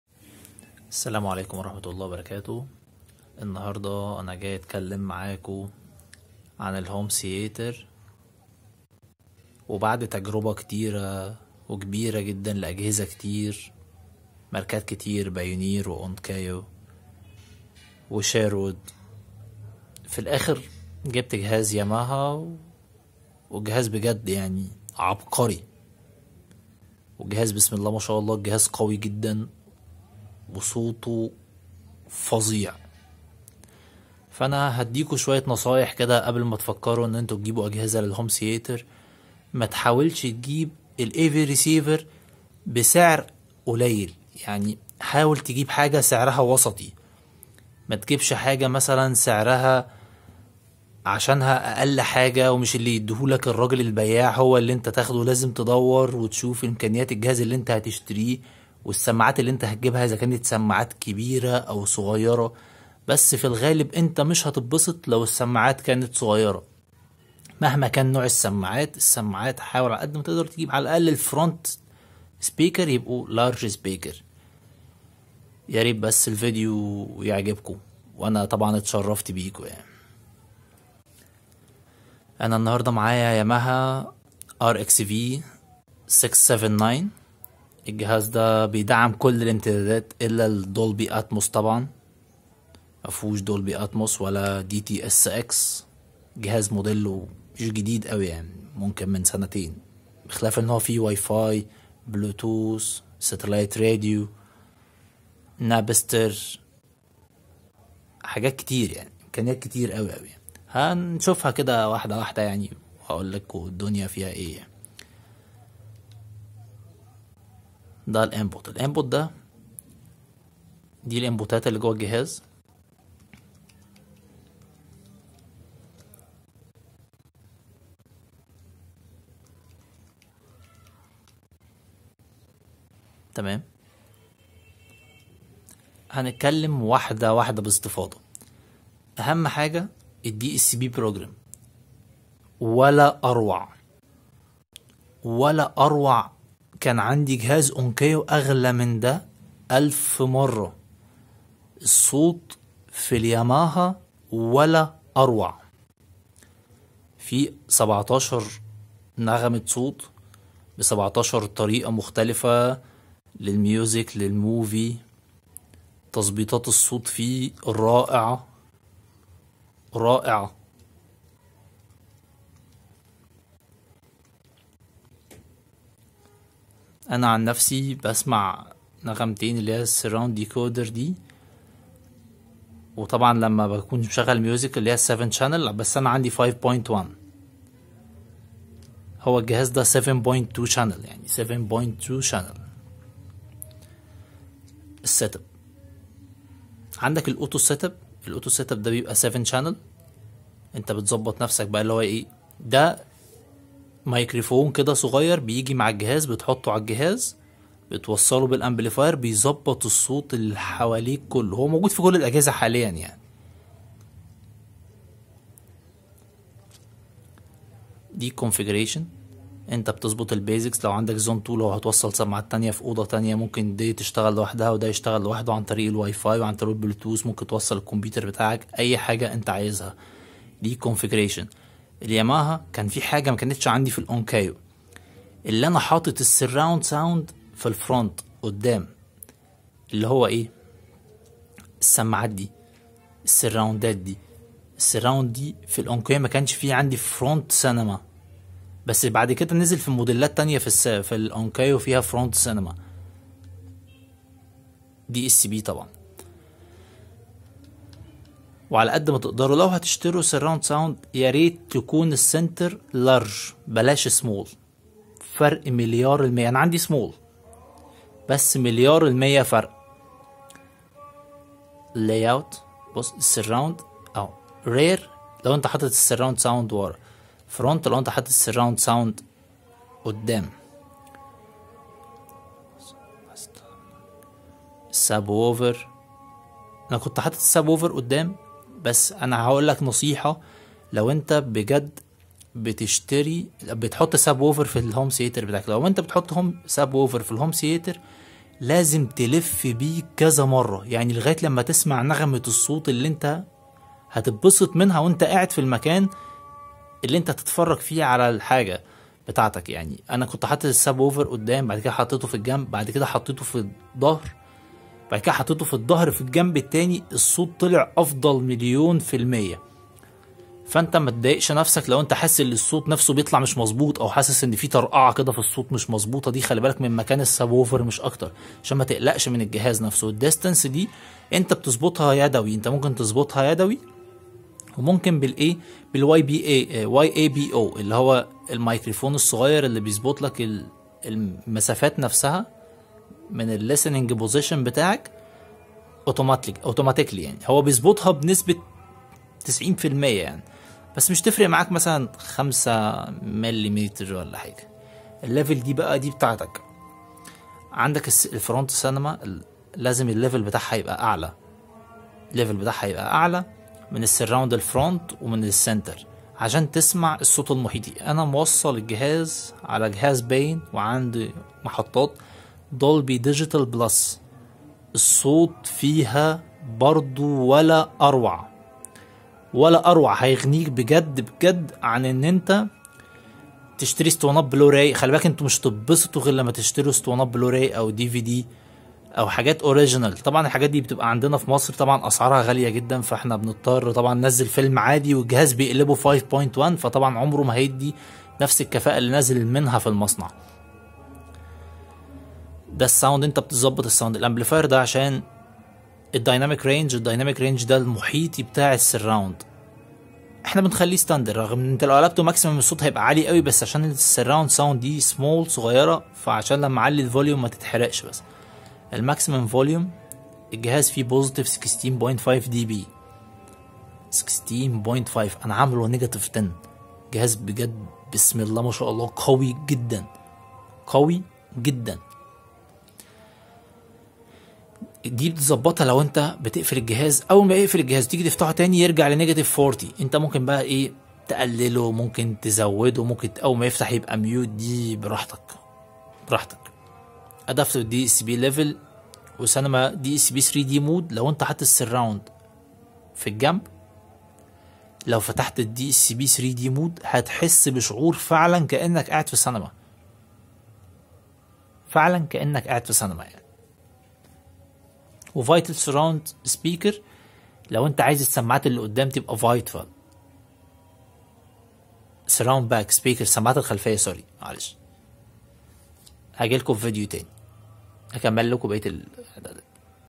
Setup menu & quick menu of the yamaha rx-v679 Network A/V Receiver
yamaha rx-v679 -ns-f150-ns-p150